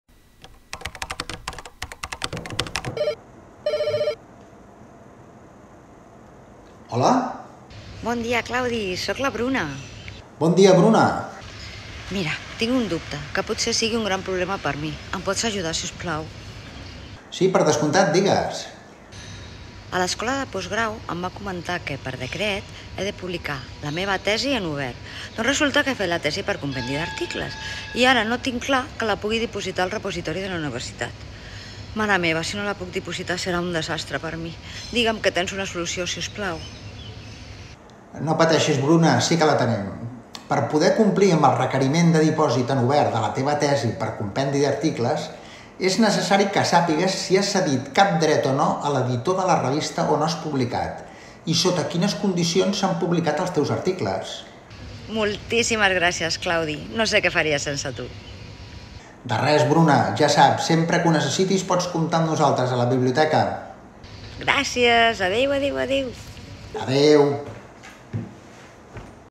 Dipòsit de la tesi per compendi de publicacions
Conversa entre els personatges del curs Propietat intel·lectual en l'elaboració de la tesi doctoral de la UAB on el bibliotecari Claudi explica a la doctoranda Bruna que per complir el requisit de dipositar en obert la tesi per compendi d'articles ha d'esbrinar quins drets ha cedit a l'editorial de la revista on ha publicat i en quines condicions s'han publicat els articles.